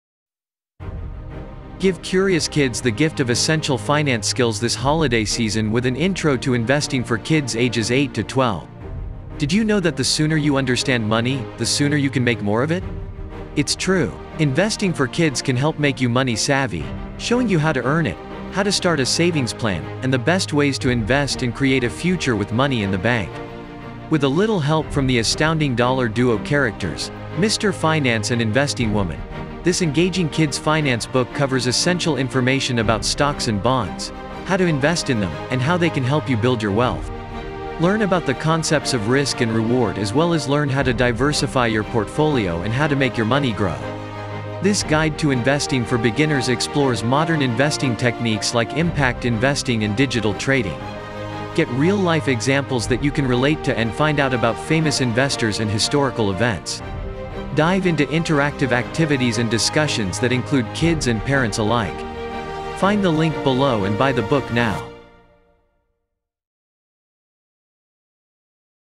Investing for Kids: How to Save, Invest, and Grow Money
Give curious kids the gift of essential finance skills this holiday season with an intro to investing for kids ages 8 to 12.

Did you know that the sooner you understand money, the sooner you can make more of it? It's true! Investing for Kids can help make you money savvy, showing you how to earn it, how to start a savings plan, and the best ways to invest and create a future with money in the bank.

With a little help from the astounding Dollar Duo characters—Mr. Finance and Investing Woman—this engaging kid's finance book covers essential information about stocks and bonds, how to invest in them, and how they can help you build your wealth. Learn about the concepts of "risk" and "reward" as well as learn how to diversify your portfolio and how to make your money grow.

Practical advice—This guide to investing for beginners explores modern investing techniques like impact investing and digital trading.

Finance 101 for kids—Get real-life examples that you can relate to and find out about famous investors and historical events.

Taking stock—Dive into interactive activities and discussions that include kids and parents alike.

This ultimate money book for kids gives you a jump-start on how to be a smart investor.